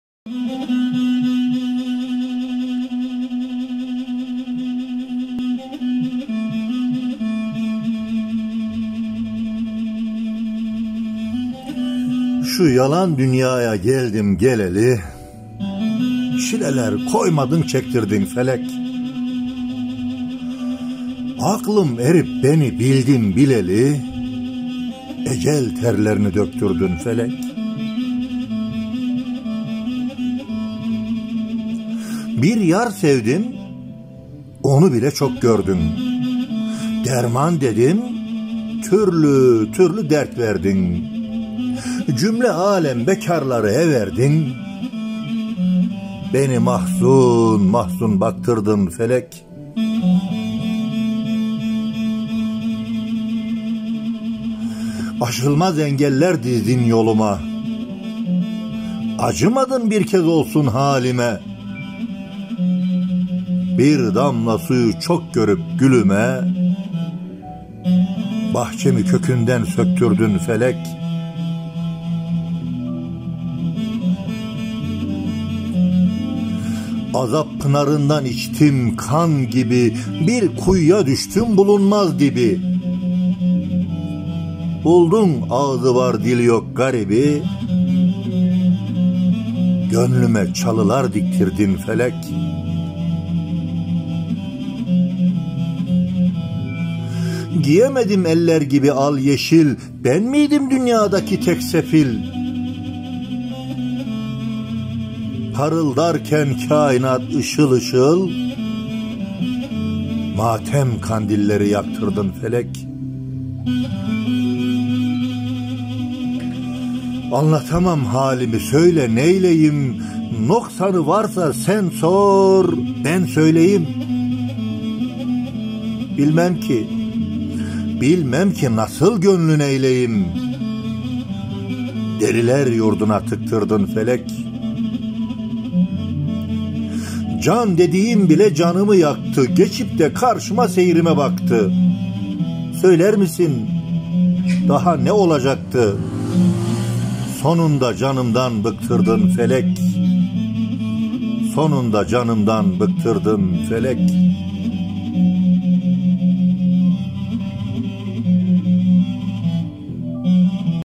Şikayet
ŞİKAYET

Şu yalan dünyaya geldim geleli
Çileler koymadın çektirdin  felek.
Aklım erip beni bildim bileli
Ecel terlerini  döktürdün felek.

Bir yar sevdim onu bile çok gördün
Derman dedim türlü türlü dert verdin
Cümle âlem bekarları everdin,
Beni mahzun mahzun baktırdın felek.

Aşılmaz engeller dizdin yoluma
Acımadın bir kez olsun halime
Bir damla suyu çok görüp gülüme
Bahçemi kökünden söktürdün felek.
Mustafa KIYAK